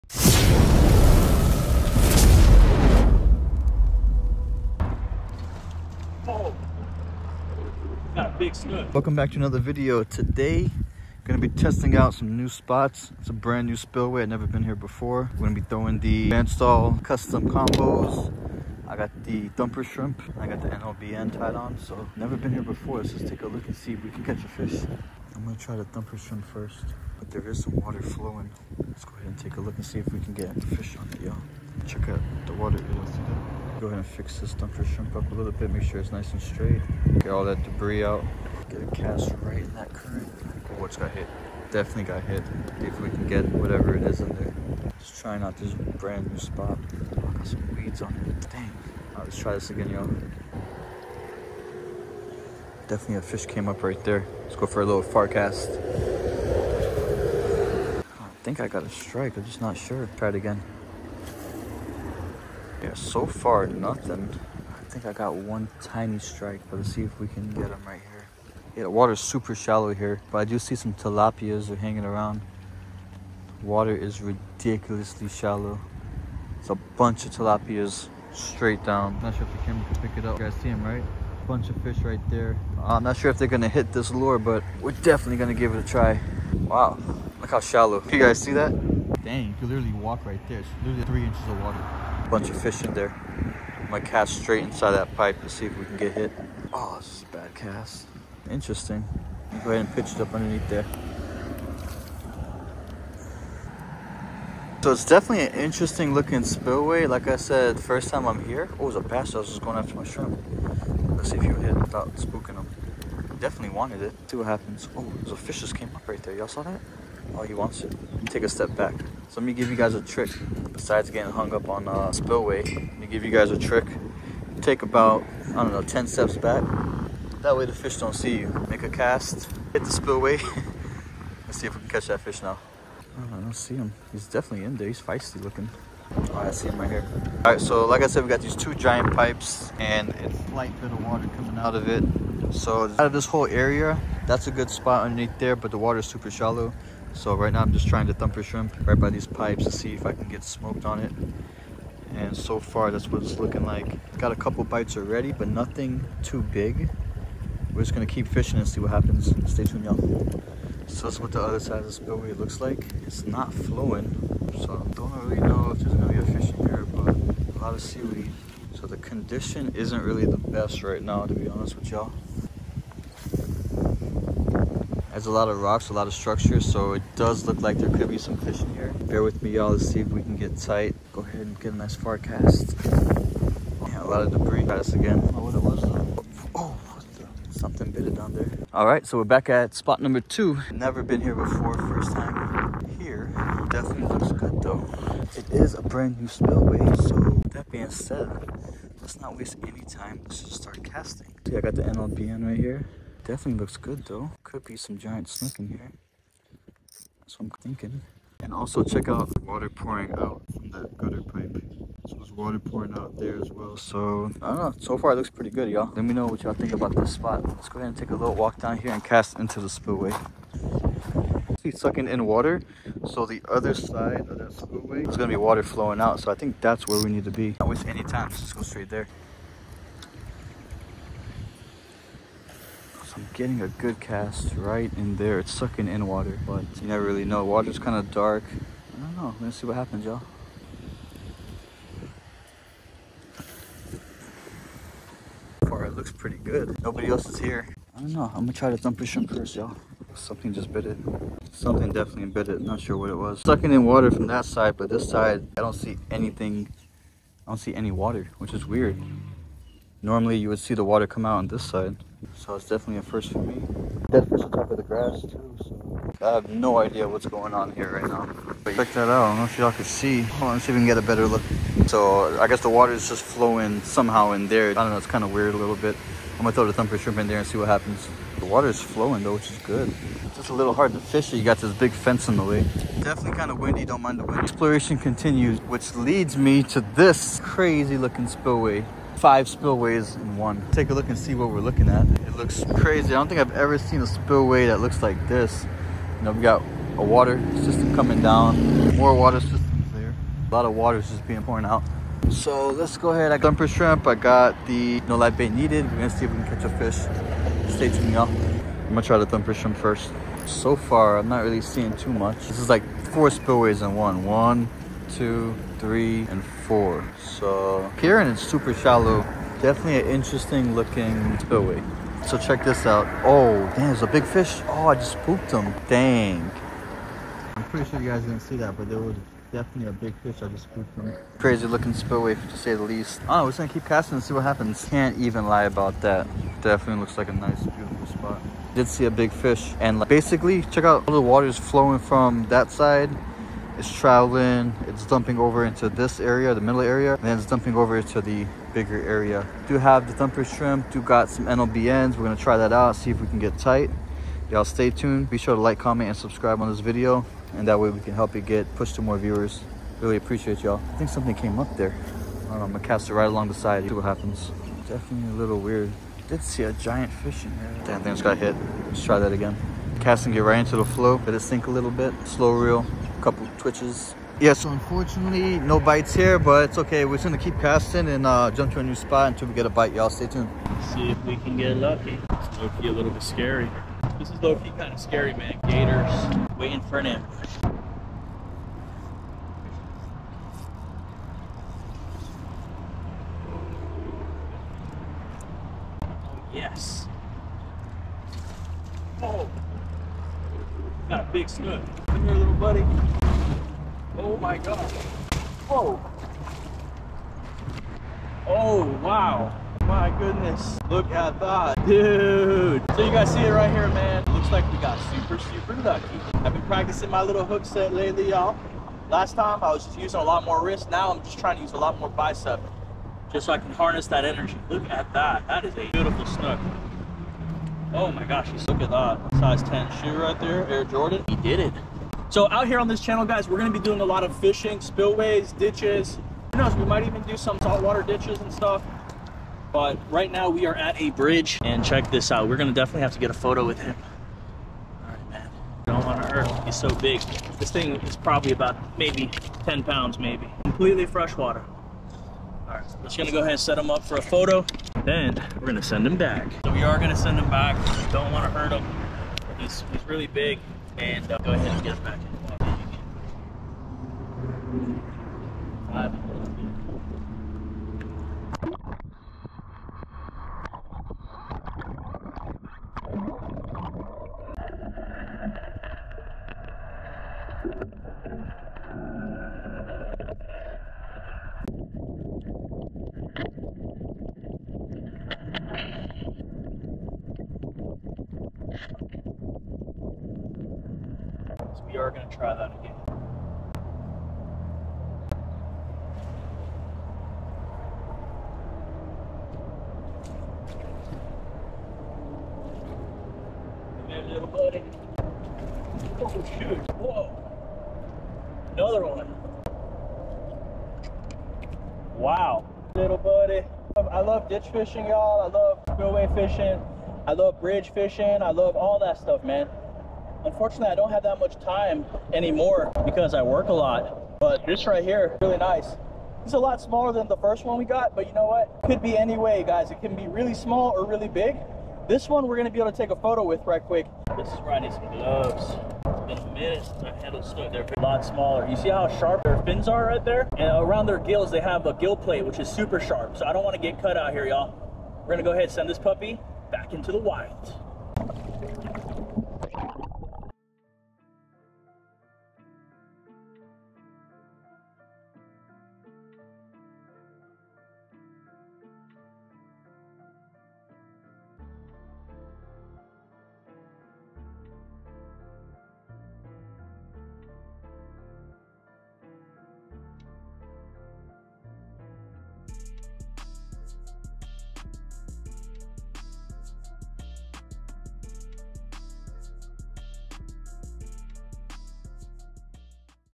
Nlbn 3” paddletail catches big snook! Discovering new spillway fishing spots in Florida !
You never know what you can catch Fishing new Spillway and bridges, New adventure equals new opportunity. Small swimbaits are highly effective for catching snook !

If you enjoy watching, be sure, give a 👍